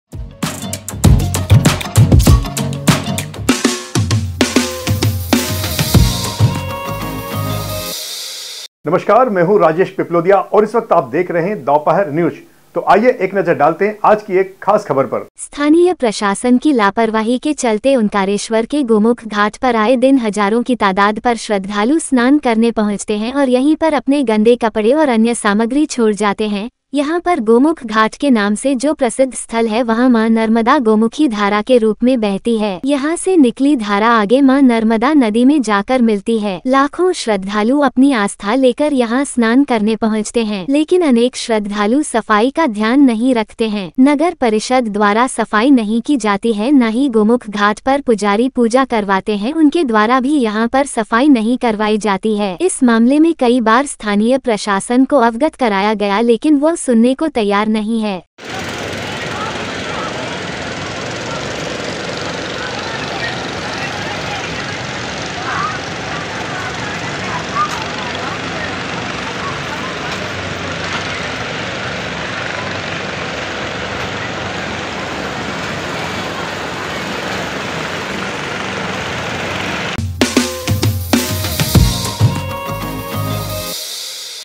नर्मदा में रोज फेंकते हैं ढेरों गंदगी गोमुख घाट की भारी दुर्दशा Dophar News
आस्था के साथ भारी खिलवाड़
गोमुख घाट पर फैली भारी अव्यवस्था
नगर परिषद ने नहीं लगाए चिन्हित बोर्ड
श्रद्धालु स्नान के बाद वहीं फेंकते हैं गंदगी व कपड़े
मां नर्मदा लगातार हो रही दूषित